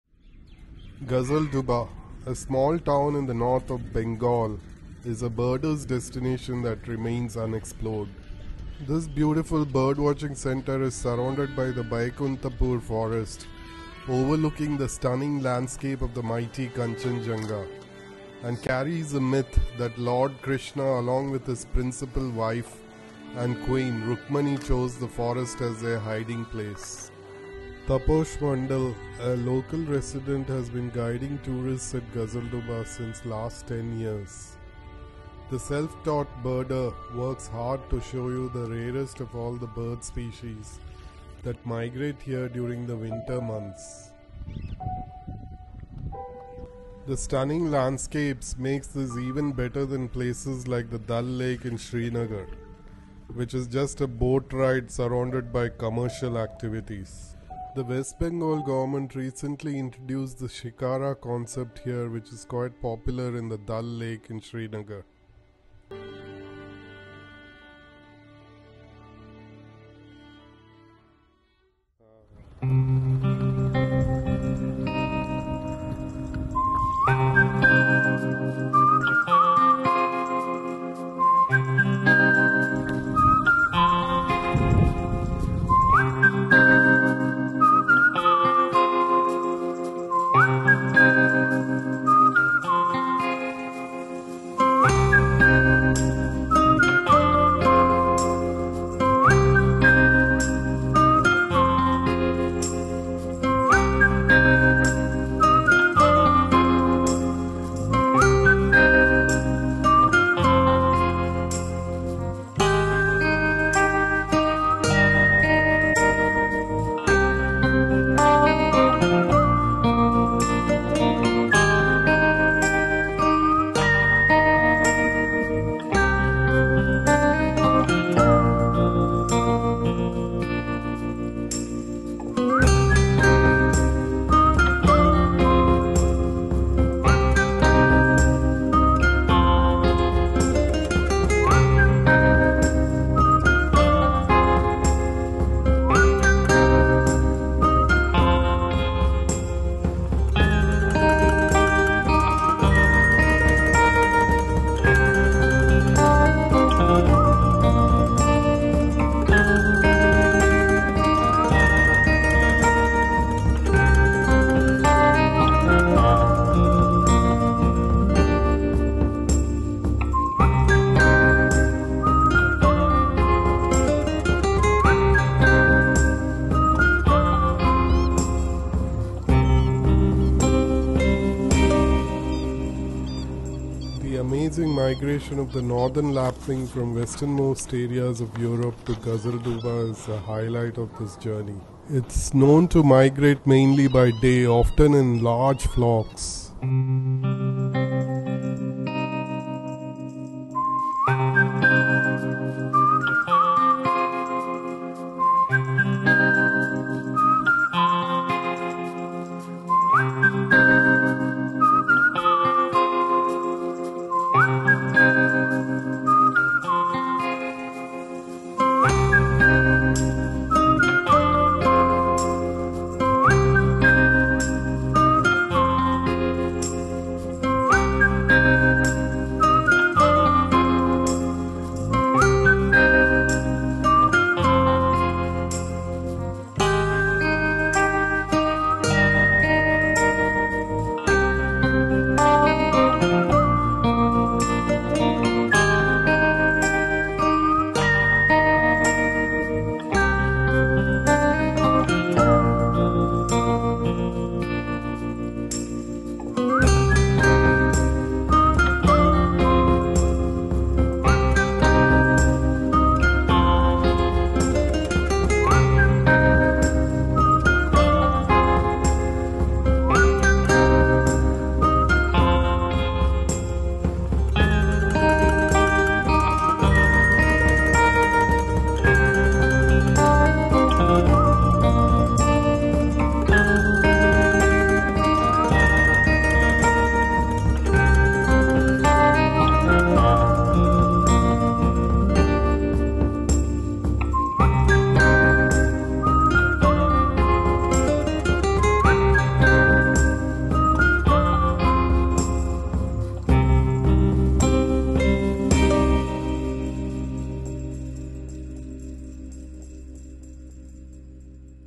Gazaldoba A Birders Paradise
Gazaldoba ~ The foothills of the Himalayas are go to destination for wildlife. Here is another great place in the foothills of the Himalayas located in the North of Bengal . This incredible place is far better than even the Dal Lake (Haven’t really understood what the fuss is all about in Srinagar) Just another place surrounded with commercial activities

Gazaldoba on the other hand provides an amazing experience for the true nature lover with the mighty Kanchenjunga staring at you while you finish an action packed birding in two sessions in a day and head back to your hotel to taste some amazing Bengali Cuisine from the North of Bengal

This was my second visit early this year with my friend and guide Tapas Mondal

This guy is the example for India and wildlife a self taught birder who can tell you all about the birds that visit here during the winter months

According to the Telegraph 70 species of migratory birds have been recorded here which is incredible